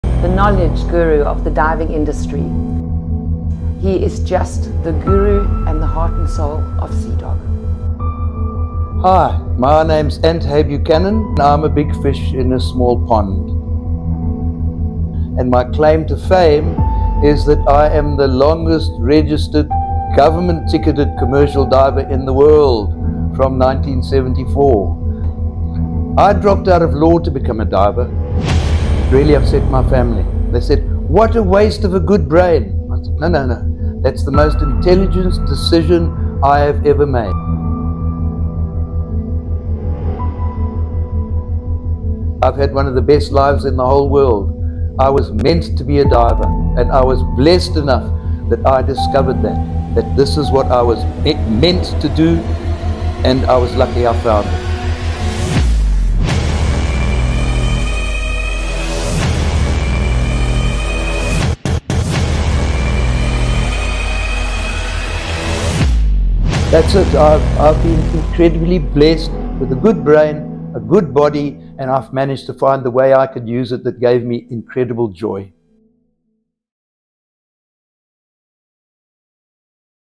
SEADOG: The life of Anthony Hay-Buchanan
Celebrating Anthony Hay Buchanan. SEADOG senior instructor, guru, legend, friend.  Started diving 1961. Commercial diving in 1965 and offshore North Sea 1976. Now celebrated by the SEADOG Family as he continues to change lives throughout the diving industry worldwide.